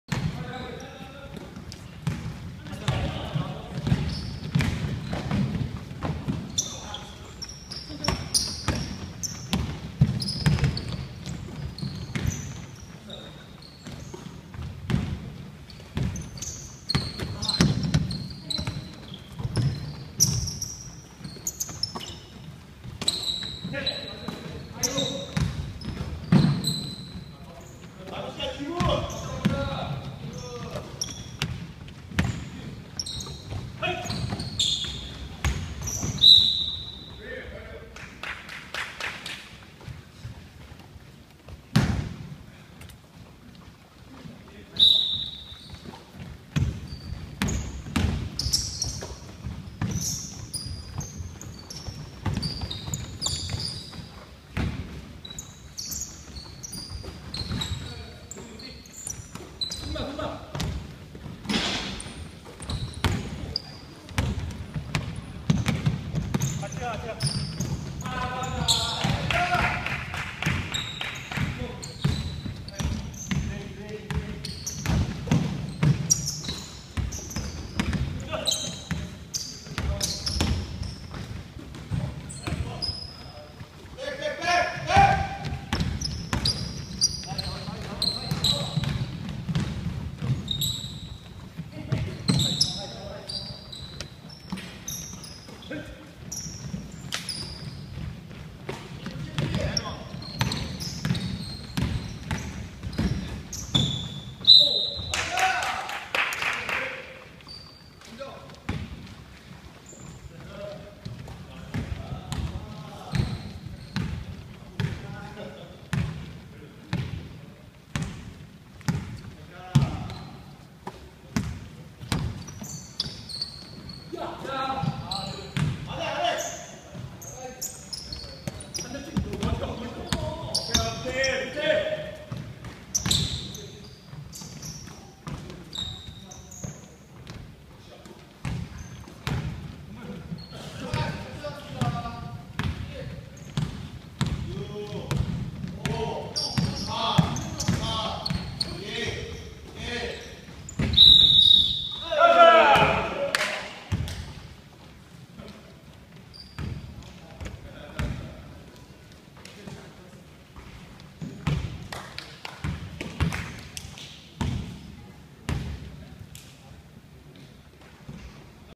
☆수도권 농구 동호회☆ 말사모 vs 러쉬 - 1경기 1쿼터 (2015.09.06) !
(KPOP Asia Basketball) South Korea HOT Streetball Game !

사회인 농구동아리 / 농구동호회 M.워리어스(말사모) vs RUSH(러쉬)

농구동호회 / 농구동아리 / 농구동영상

독산고등학교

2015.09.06 in Korea, Seoul (대한민국,서울)